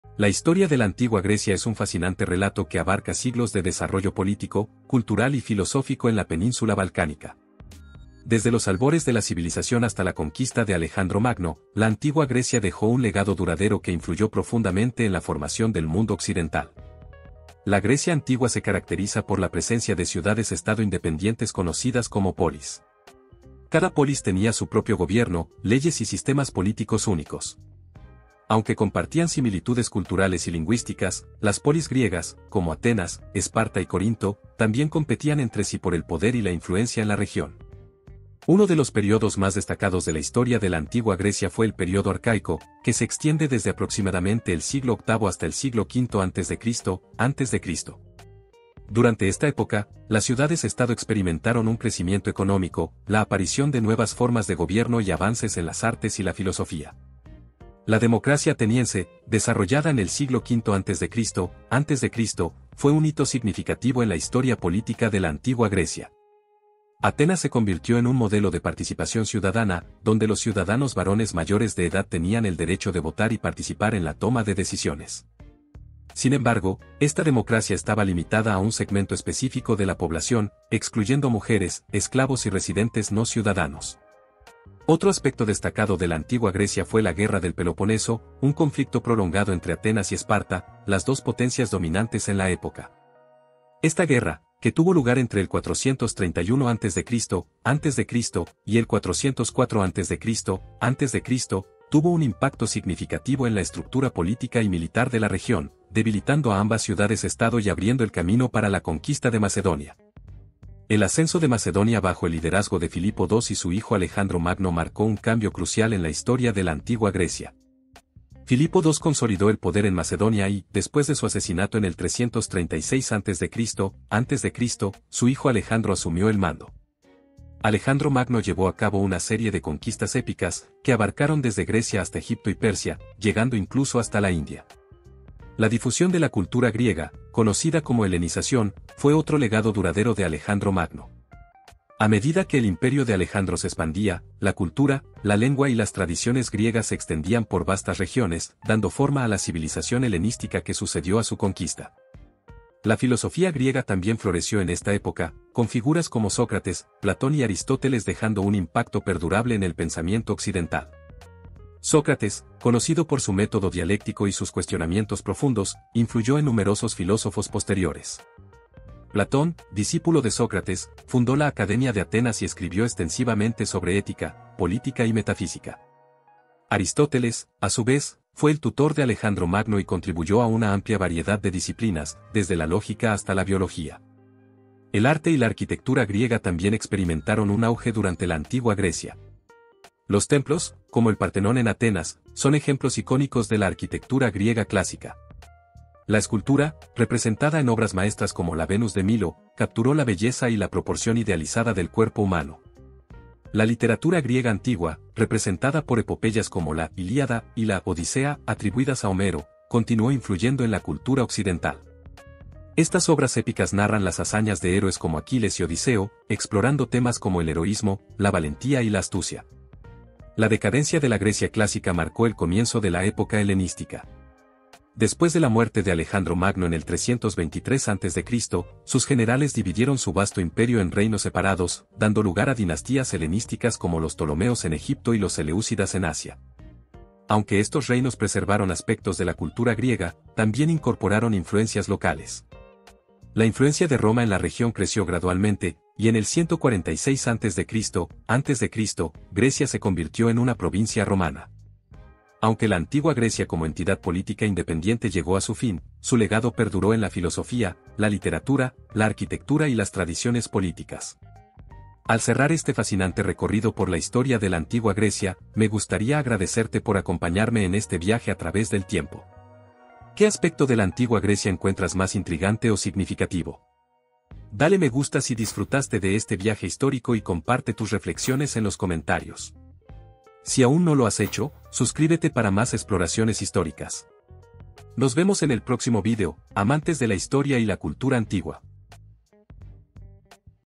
Legados de la Antigua Grecia: Mitos, Democracia y Sabiduría
Explora el fascinante mundo de la Antigua Grecia a través de sus mitos legendarios, el nacimiento de la democracia y la búsqueda incesante de conocimiento y sabiduría. Sumérgete en una civilización que ha dejado un legado perdurable en la historia, donde la filosofía, el arte y la política se entrelazan en un relato épico de grandeza y humanidad.